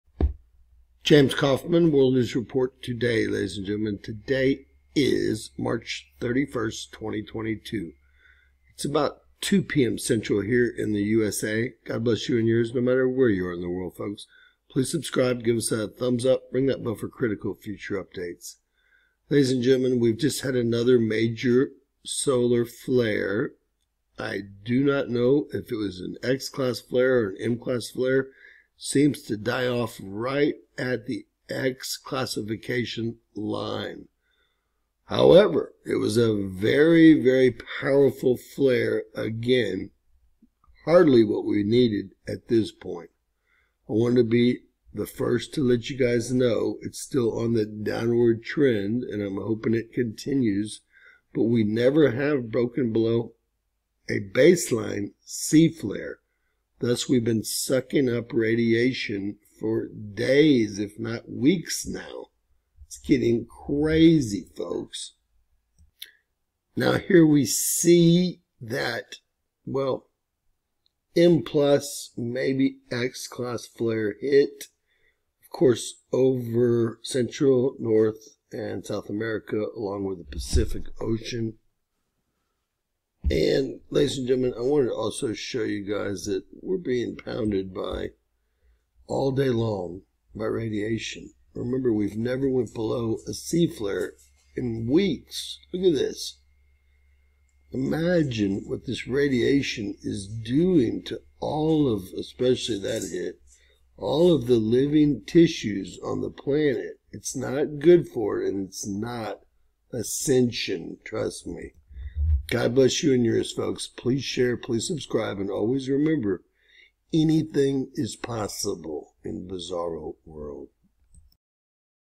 Another Large Solar Flare And CME Inbound March 31st 2022!
Share on FB and Twitter if you are able and help the channel if you are able..